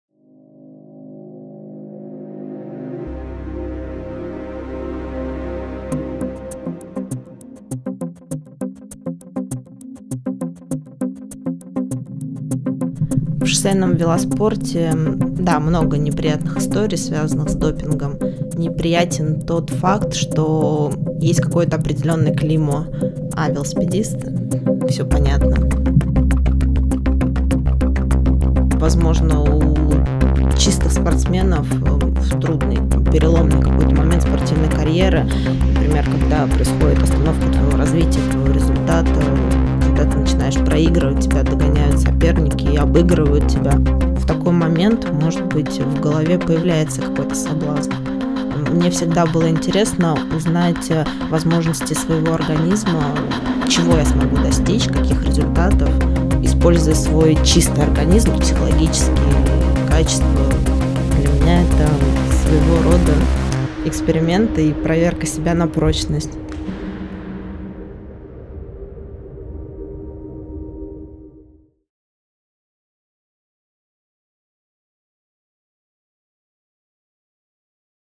Истории спортсменов: Анастасия Войнова о выборе между допингом и возможностями чистого организма!
Честный рассказ участника проекта Siberian Wellness "Натуральная правда" серебряного призера Олимпийских игр-2016 по велоспорту на треке в командном спринте Анастасии Войновой о возможностях чистого организма и допинга.

"В велоспорте много неприятных историй, связанных с допингом. Неприятен тот факт, что есть какое-то определенное клеймо: "А велосипедист? Все понятно!".

А ты – за допинг или против?

Таймлайн:
0:12 Допинг в велоспорте
0:28 Анастасия Войнова против допинга